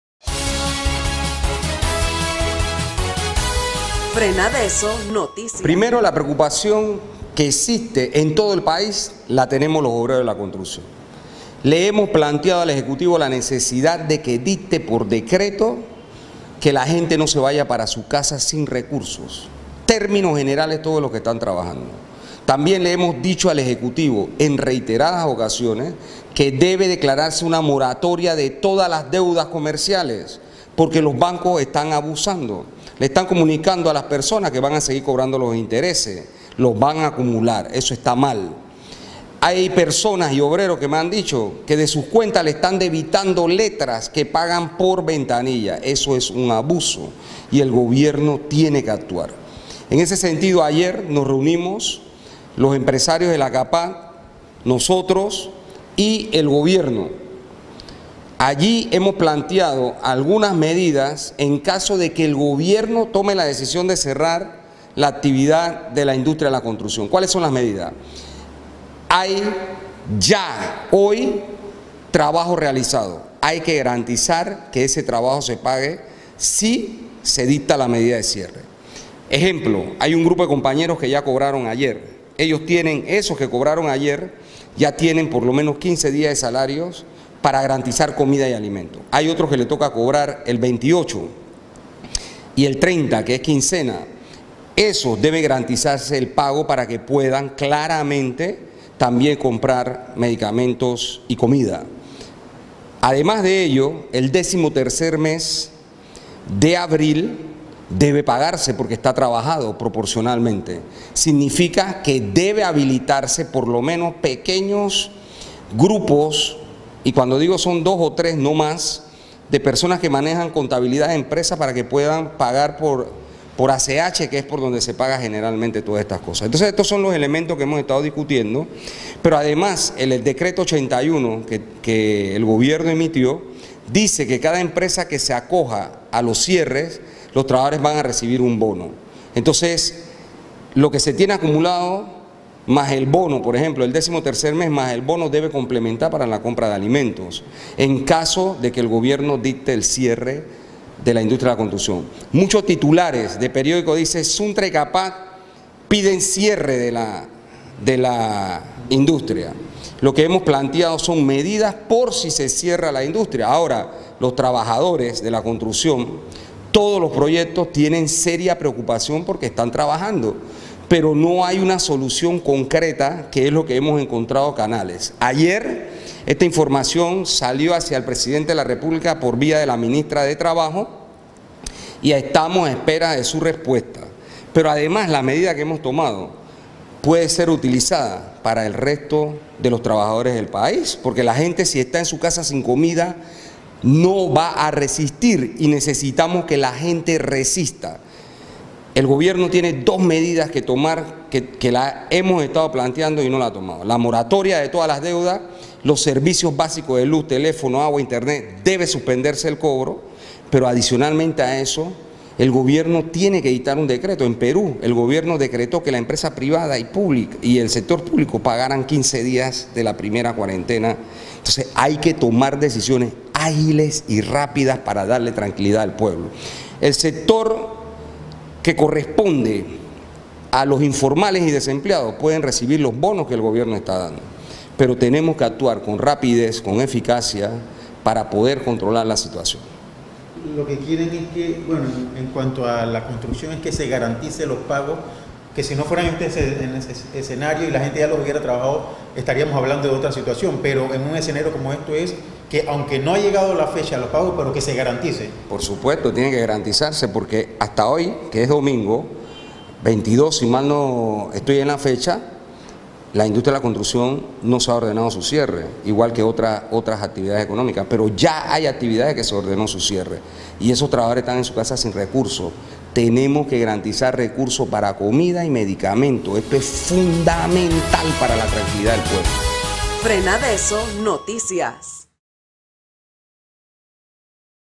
Entrevista Saúl Méndez Telemetro Panamá 2232020 Merge1
Pago de salarios para trabajadores en caso de que gobierno ordene cierre de los proyectos. . .